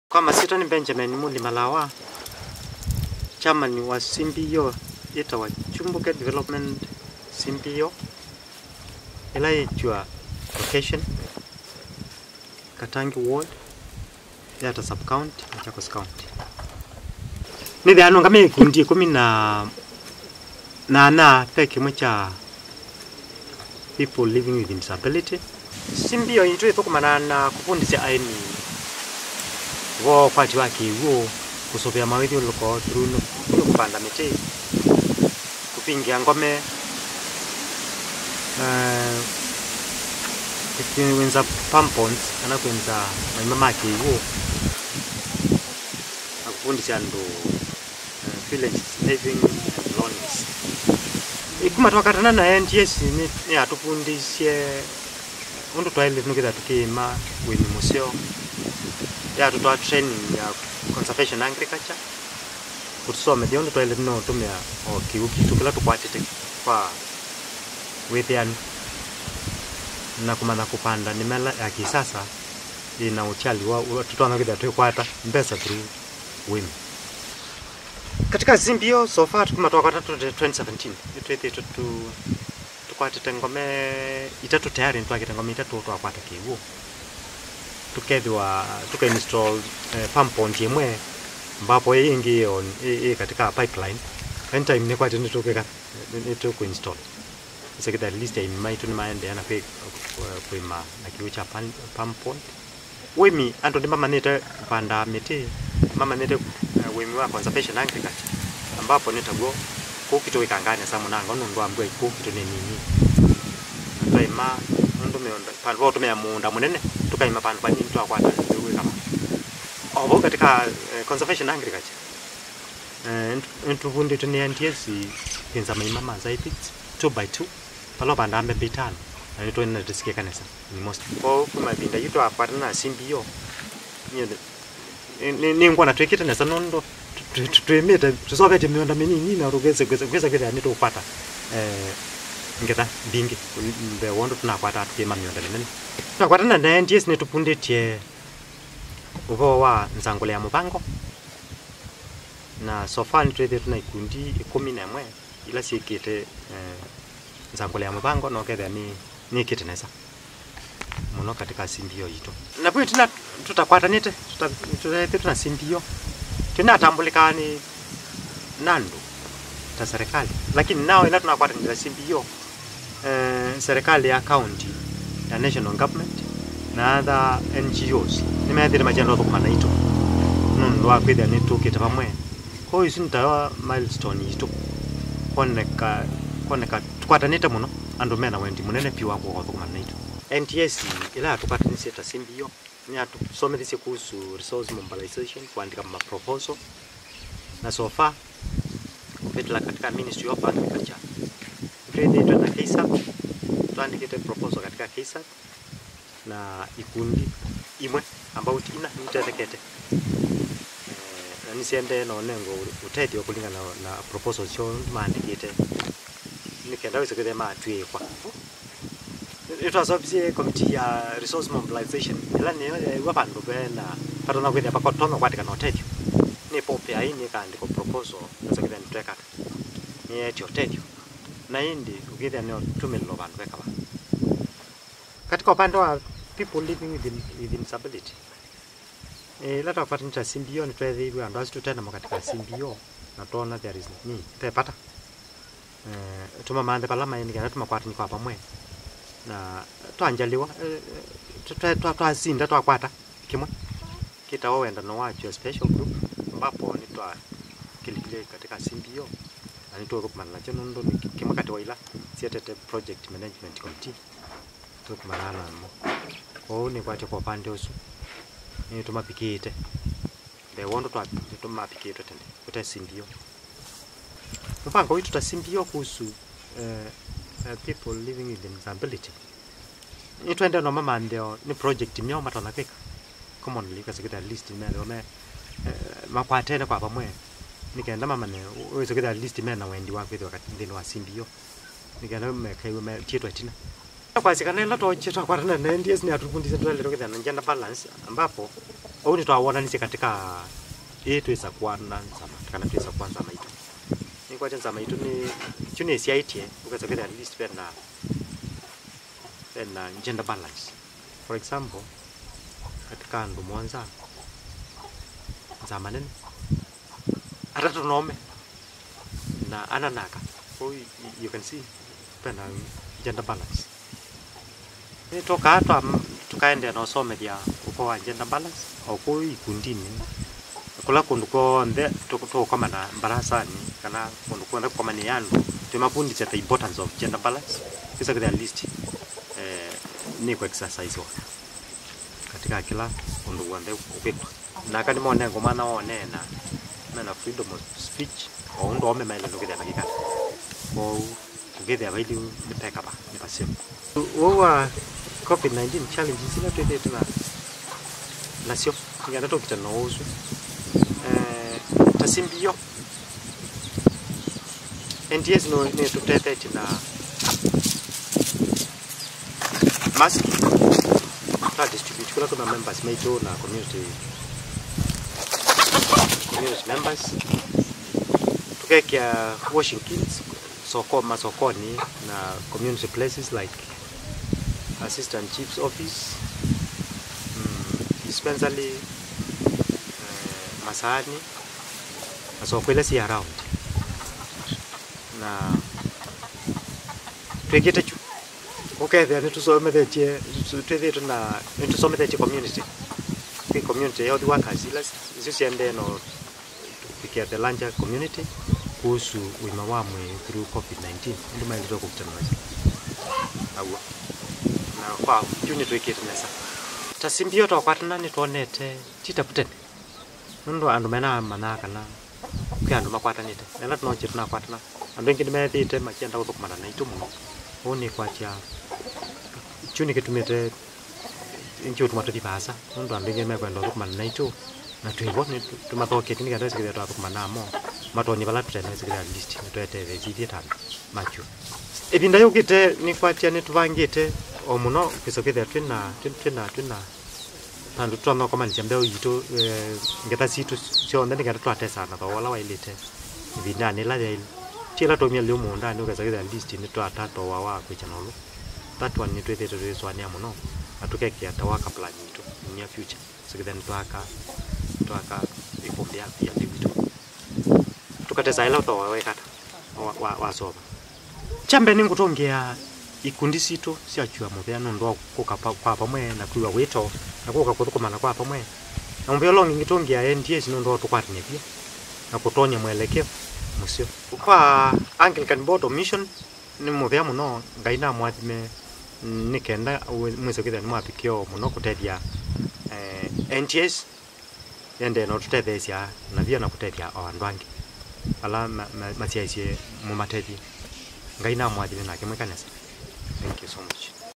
The Kyumbuke CBO in Kyua Location, Machakos County.
A vibrant organization transforming communities to enjoy secure and sustainable livelihoods through participatory approaches and Godly Principles for quality life.